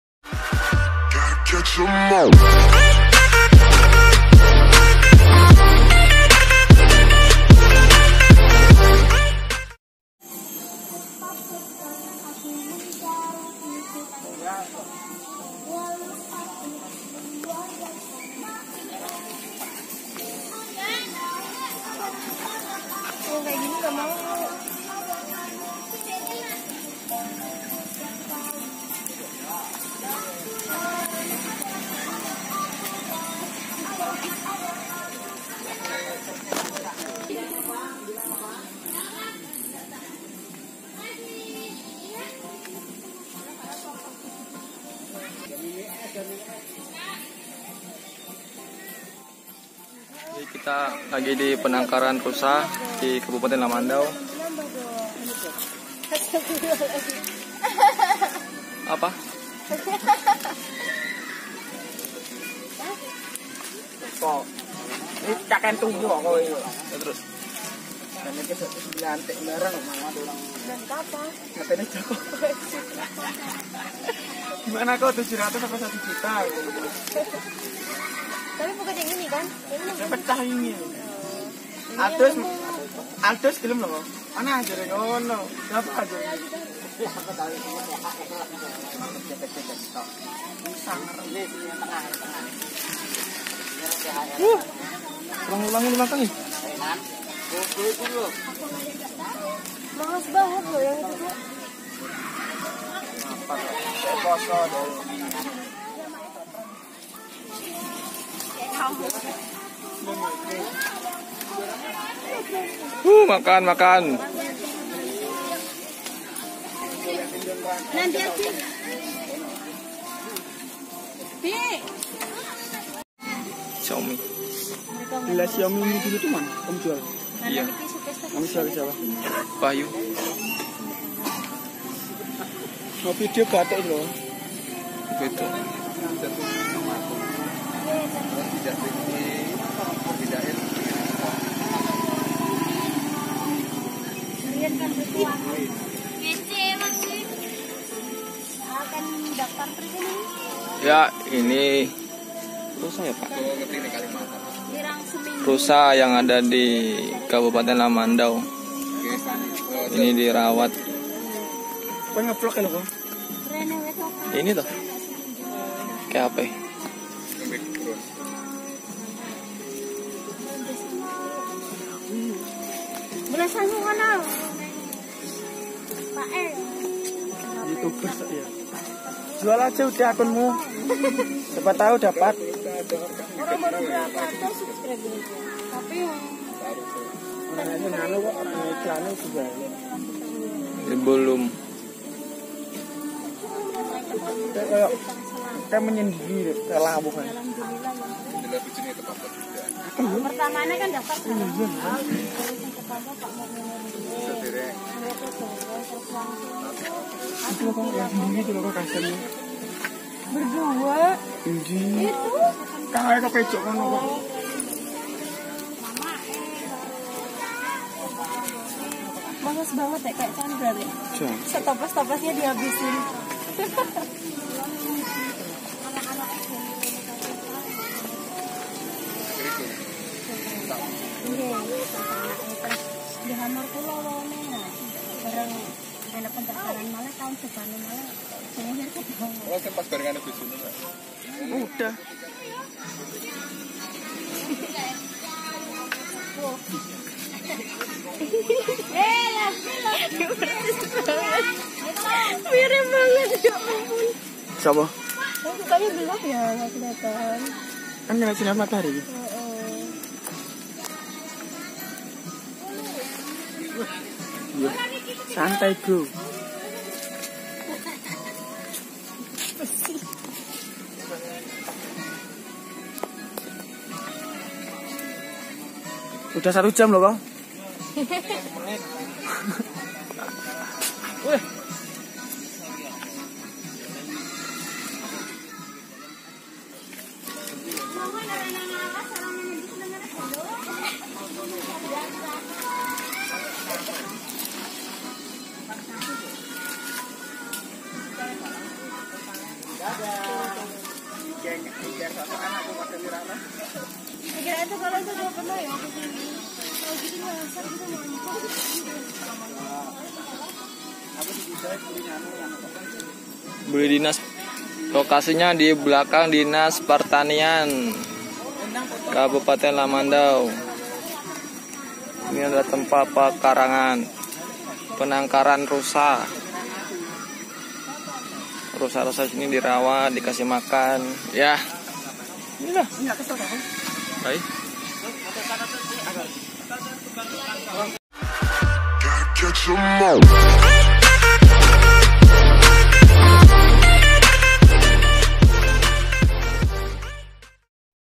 PENANGKARAN RUSA DI NANGA BULIK, "KABUPATEN LAMANDAU"
Penangkaran rusa yang ada di NANGA BULIK yang merupakan salah satu simbol dari kabupaten Lamandau.